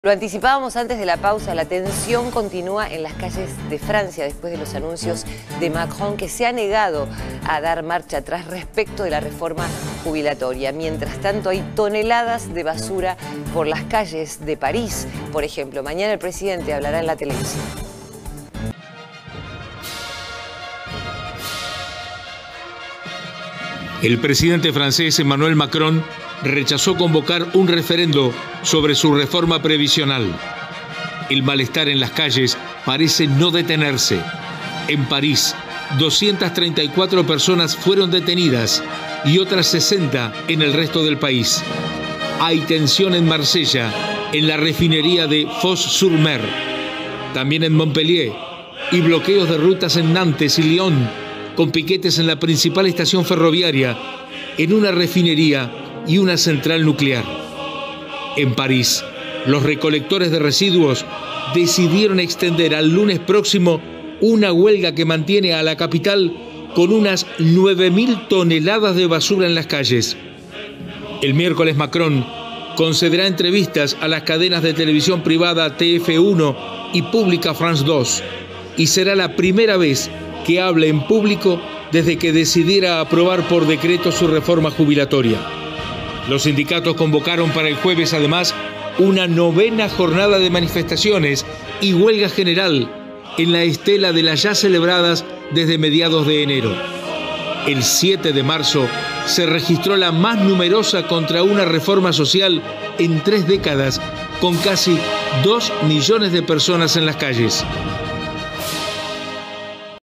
Emmanuel Macron rechazó un referéndum y defenderá su reforma previsional en televisión abierta
Por su parte, los sindicatos convocaron para el jueves una novena jornada de manifestaciones y huelga general.